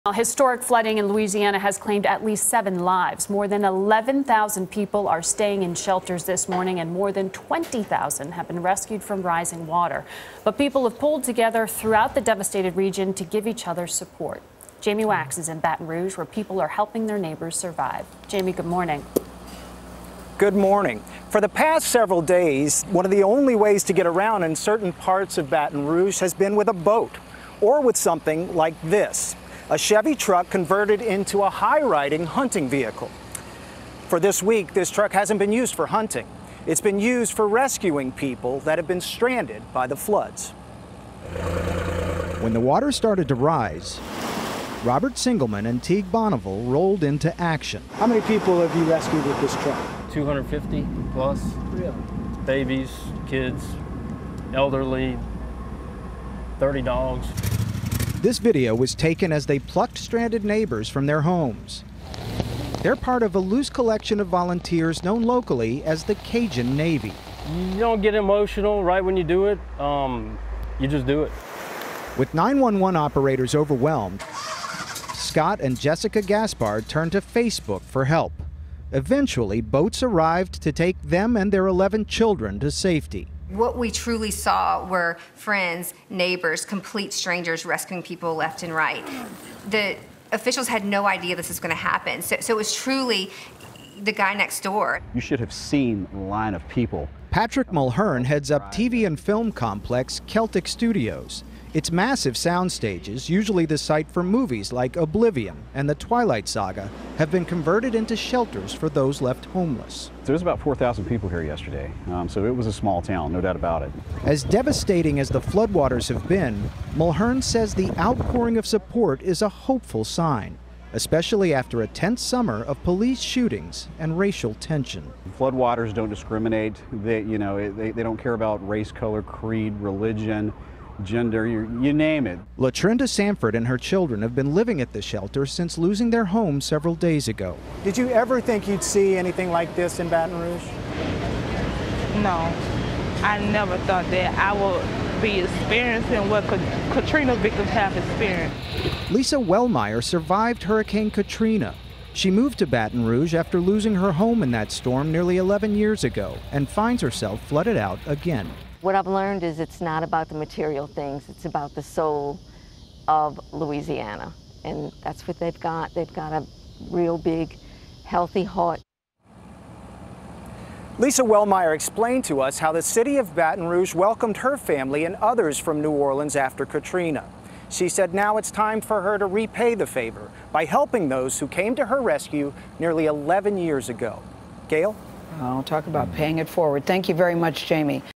Louisiana flood victims turn to each other to survive crisis
Historic flooding in Louisiana has claimed at least seven lives. More than 11,000 people are staying in shelters, and more than 20,000 have been rescued from rising water. But people have pulled together throughout the devastated region to give each other support. Jamie Wax reports on an army of volunteers known locally as the "Cajun Navy."